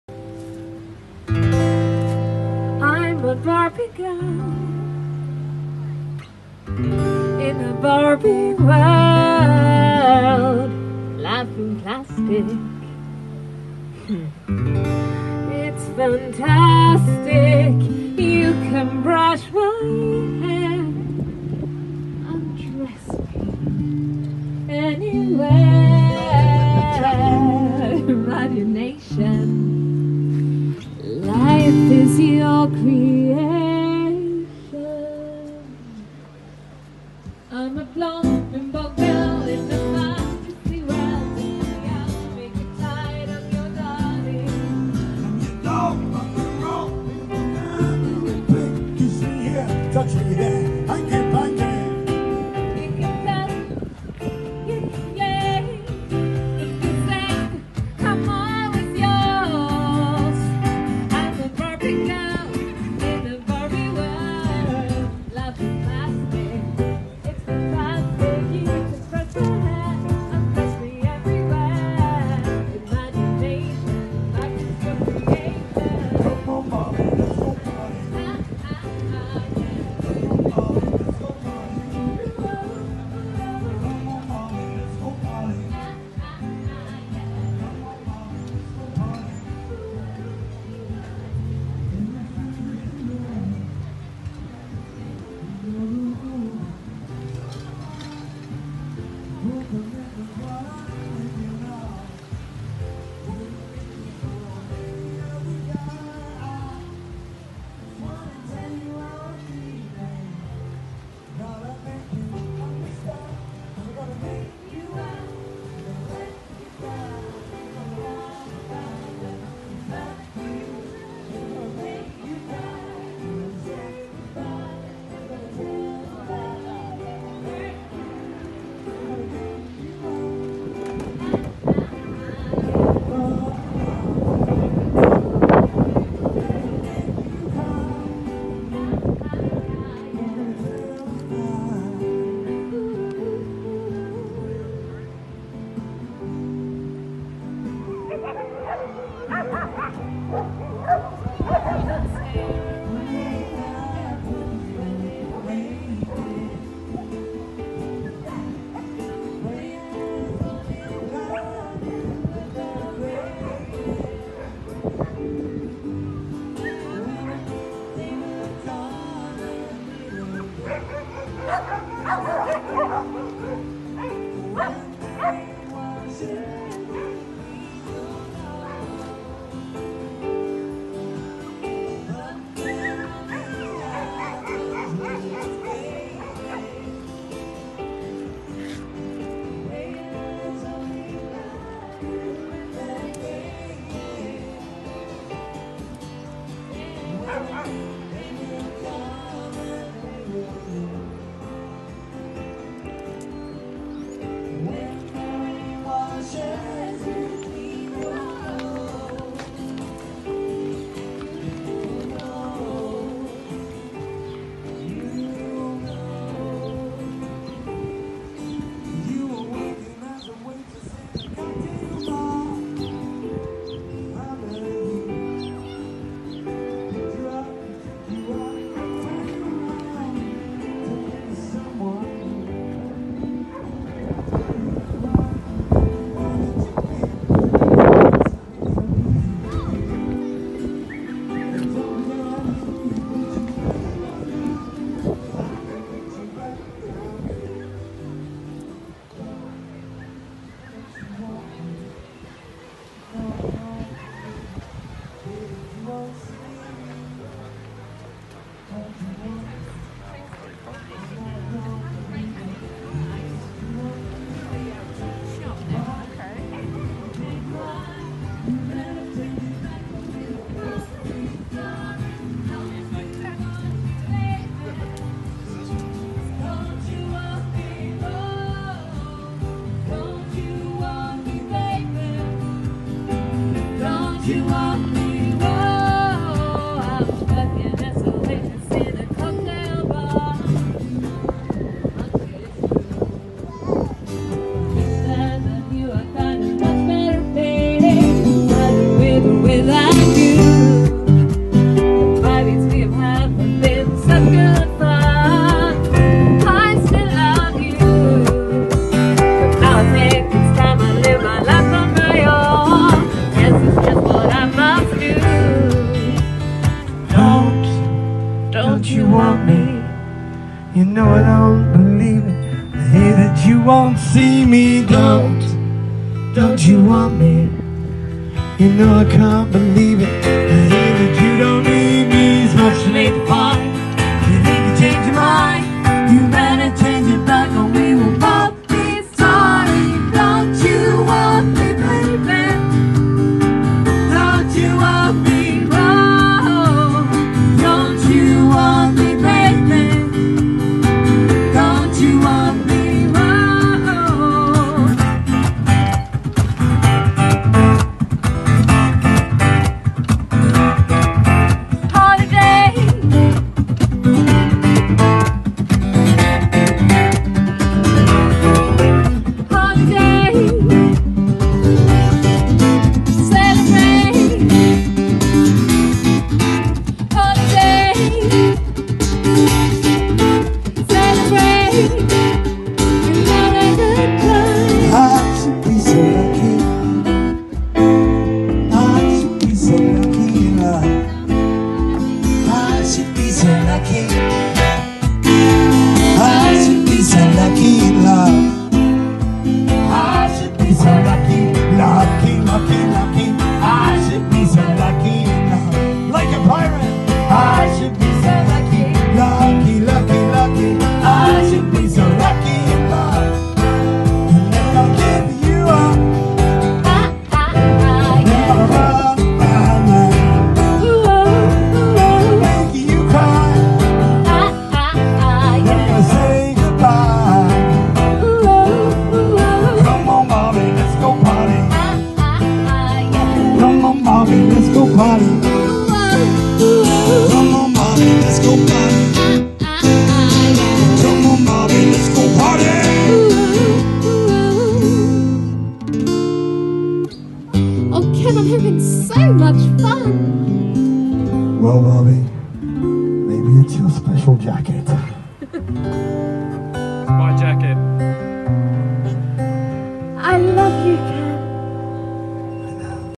The Awesome Medley, Live at the Anchor Inn, Beer...m
Saturday 28th July - a day of heavy showers, hence the lack of a crowd, but we braved it and had fun anyway! Always love it at the Anchor!